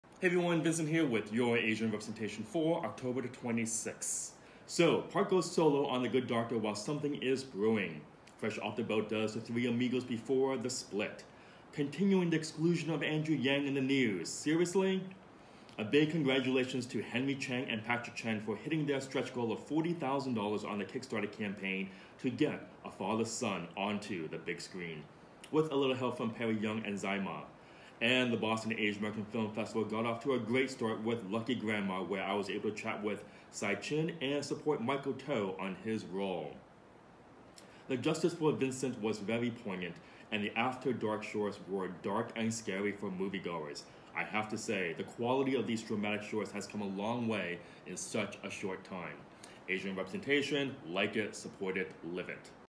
AsianRepresentation Recap - 102619
Park Goes Doctor Solo, Fresh Split, Incude Andrew Yang! Success for "A Father's Son!" Lights, Camera, Action at BAAFF! Seeing stars at Lucky Grandma, Justice for Vincent (not me), Long Lasting Shorts!